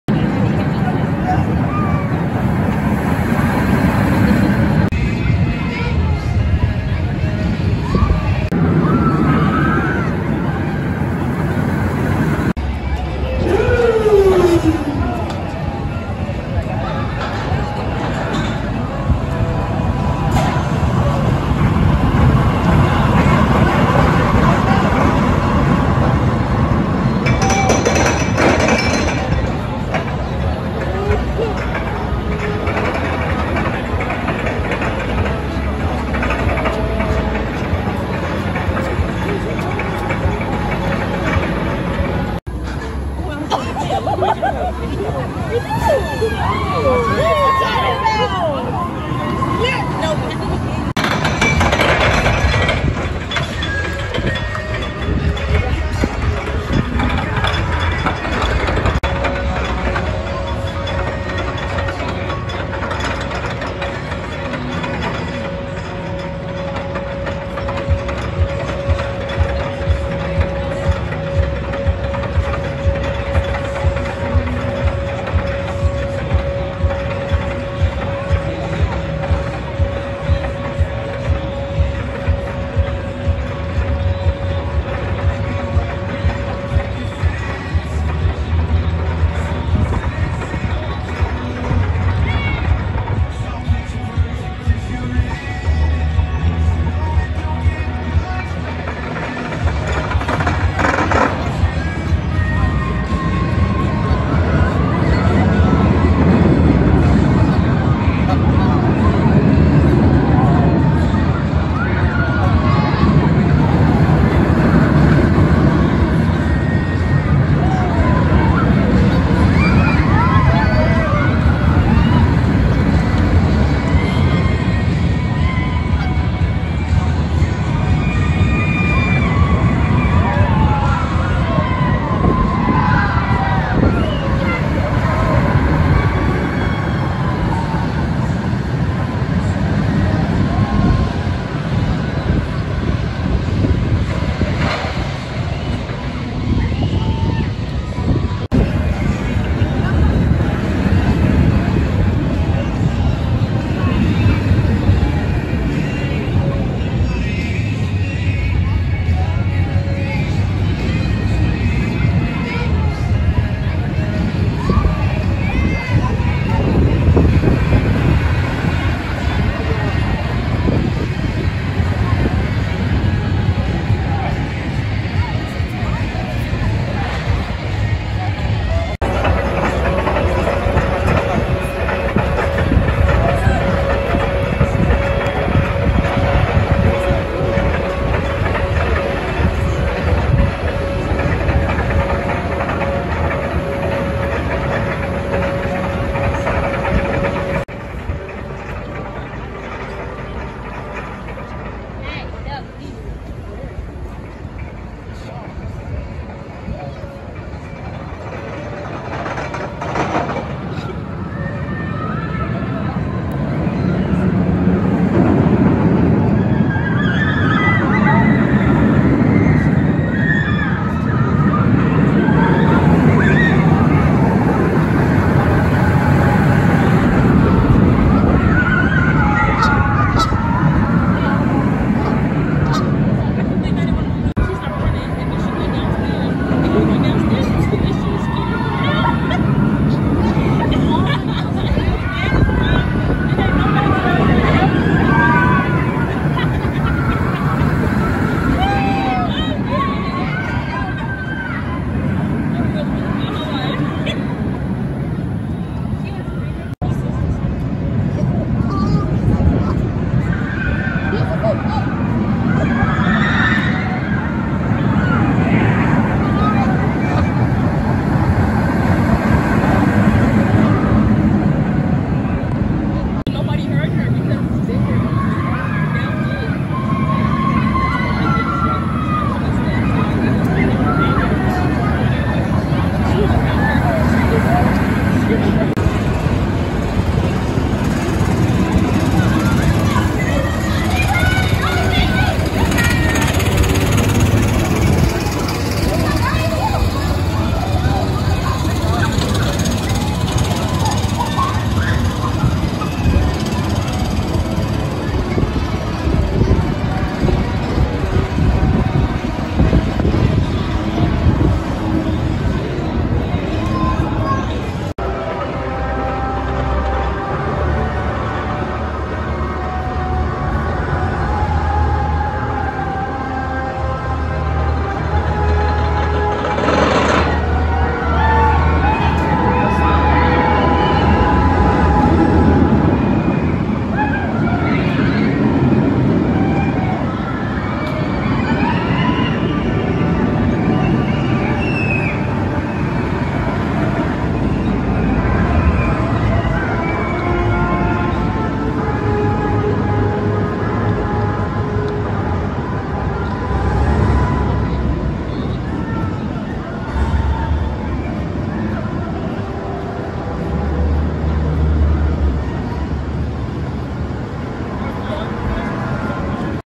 Blue Hawk @ Six Flags Over Georgia | Off-Ride
This Vekoma sit-down looping roller coaster looks beautiful traveling over the water. However, the various inversions just beat me up too much to make it truly enjoyable. I agree with a local coaster enthusiast that I met at Six Flags Over Georgia who explained this coaster as "pointless". However, I'm glad I was able to ride a ride like this, as it is a nice experience to have, even if not the most enjoyable.

รถไฟเหาะตีลังกาแบบนั่งได้ของ Vekoma นี้ดูสวยงามขณะเดินทางเหนือน้ำ อย่างไรก็ตาม การผกผันต่าง ๆ ทำให้ฉันผิดหวังมากเกินไปที่จะทำให้มันสนุกได้อย่างแท้จริง ฉันเห็นด้วยกับผู้ที่ชื่นชอบรถไฟเหาะในท้องถิ่นที่ฉันพบที่ Six Flags Over Georgia ซึ่งอธิบายว่ารถไฟเหาะนี้ "ไร้จุดหมาย" อย่างไรก็ตาม ฉันดีใจที่ได้นั่งรถแบบนี้ เพราะมันเป็นประสบการณ์ที่ดี แม้ว่าจะไม่ใช่สิ่งที่สนุกที่สุดก็ตาม

0:00 - 0:12 Intro
0:12 - 0:51 Train Leaving The Station
0:51 - 1:42 Lift Hill
1:42 - 2:28 Full Run
2:28 - 3:00 Double Inline Twist
3:00 - 3:54 Close Up Run (From Line)
3:54 - 4:45 Slightly Different Angle
4:45 - 5:15 Far Away (Through Trees)
5:15 - 6:07 Peak of Hill (Far Away)